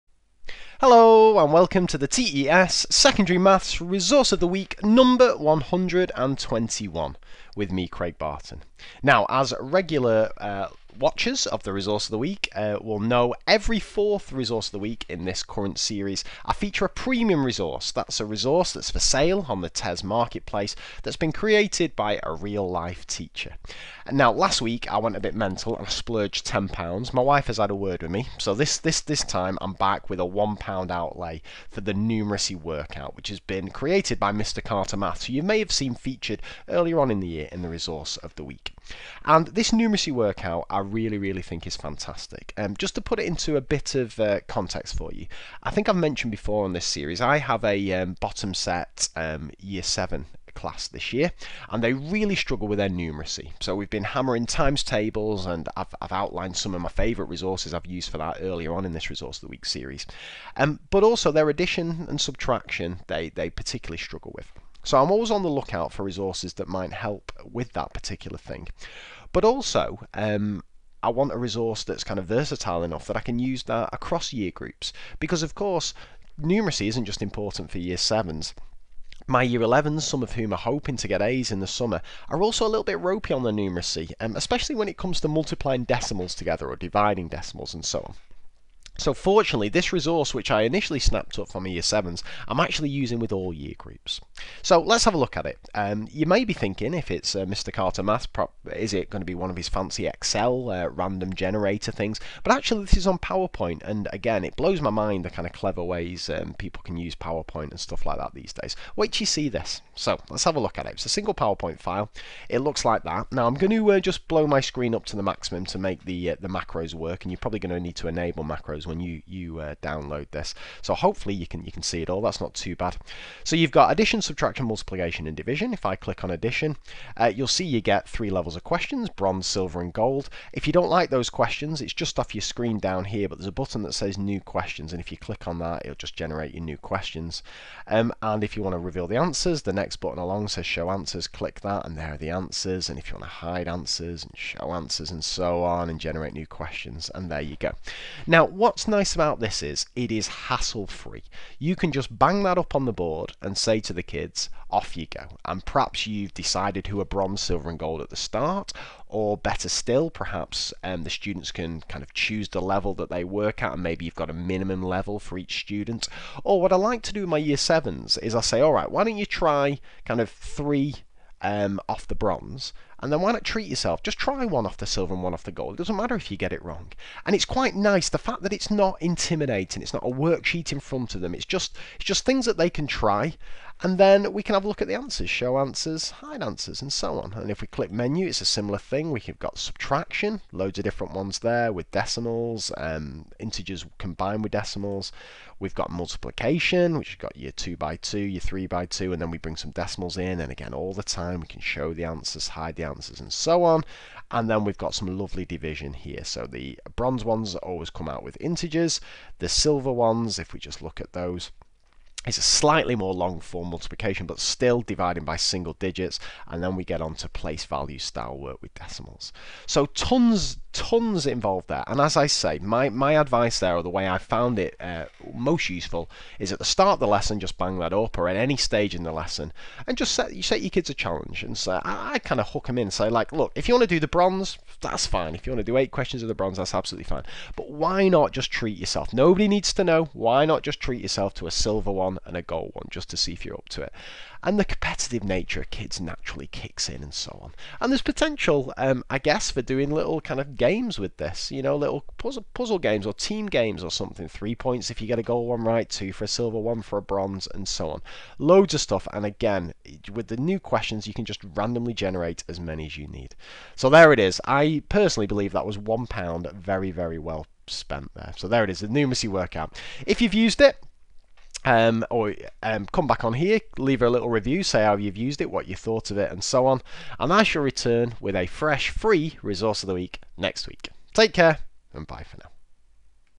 Maths ROTW 121 - Numeracy Workout video
Every fourth Resource of the Week this year will highlight a top-quality Premium resource for sale on TES that has been created by a teacher.

Sometimes I just need something very simple doing in exactly the way I want. In this instance, I wanted a clean, clear, dead easy way of allowing my students to practise their written and mental arithmetic skills. I wanted a wide range of levels of difficulty, the ability to generate as many different versions of the questions as I needed, and crucially I also wanted to answers so the students could mark them themselves. Step forward Mr Carter Maths, with another incredible use of technology to provide exactly what I needed. Via a really clever use of PowerPoint you have three levels of questions on Addition, Subtraction, Multiplication and Division, involving both integers and decimals. It is exactly what I needed.

How can it be used?
This is ideal to use at the start of a lesson so there is something on the board as the students enter the room. I have got into a routine of using it once a week with all my classes. My Year 11s had a fit when I showed it to them, but when challenged to answer the Gold questions correctly, they struggled. And of course, it is a lack of being able to do written arithmetic competently that can cost them valuable marks in their GCSE. I have seen a rapid improvement over the weeks we have been using this resource, and hopefully it will pay dividends (and certainly justify my £1 investment) come the summer!

So, in short, this is an excellent resource for all ages and abilities which will help them consolidate key numeracy skills.

Craig Barton

A word from the author
When I made this resource I had just been made Numeracy Coordinator at my last school. I needed to create an activity that form teachers across the school could use and pupils would be able to check their answers. All but those with some serious tech phobia found it really simple.

I later realised what a helpful resource this was in my own classroom, from lesson with pupils who really need to work on the basics to using it as a starter with a top set. The on screen differentiation makes sure even the most able in the group are challenged appropriately.